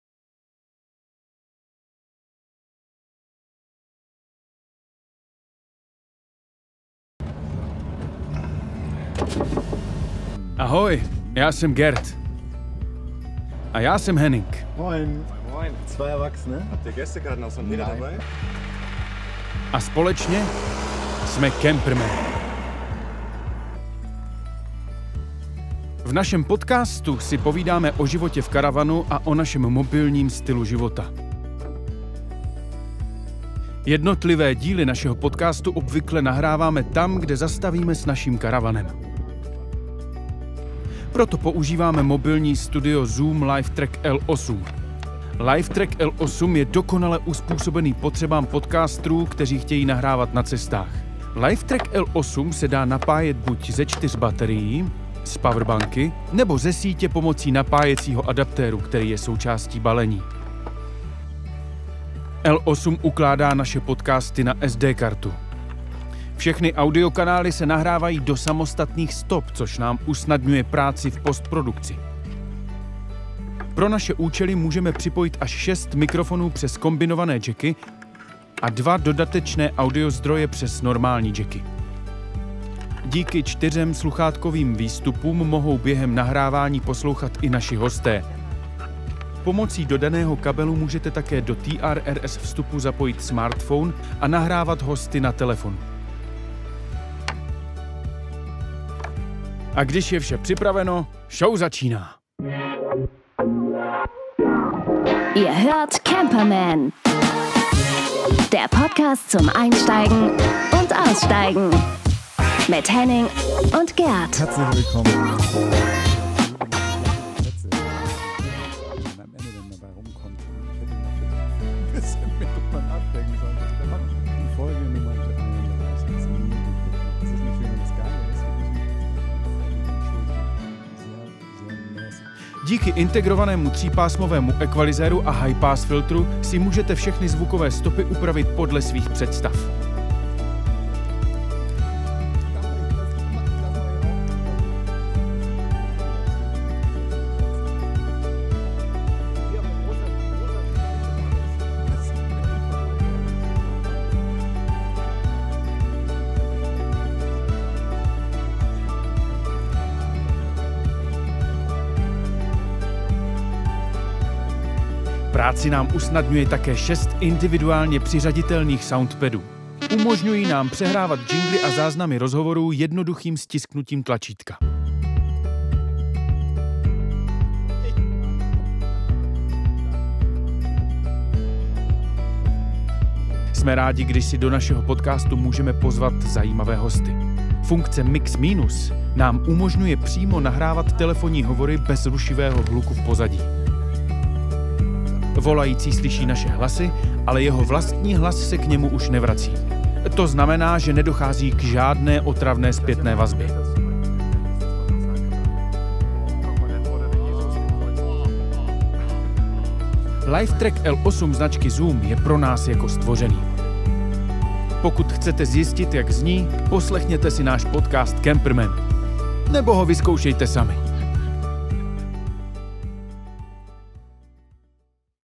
Zoom LiveTrak L-8 and The Campermen - Czech Version
Zoom LiveTrak L-8 and The Campermen
PODCASTEŘI, ZBYSTŘETE!
LiveTrak L-8 byl vyvinut speciálně pro podcastery, kteří chtějí tvořit podcasty na cestách. V tomto videu vám dvojice německých podcasterů a Zoom Creatorů Campermen ukáže, jak jim náš LiveTrak L-8 usnadňuje tvorbu podcastů na cestách.

We’re Zoom. And We’re for Creators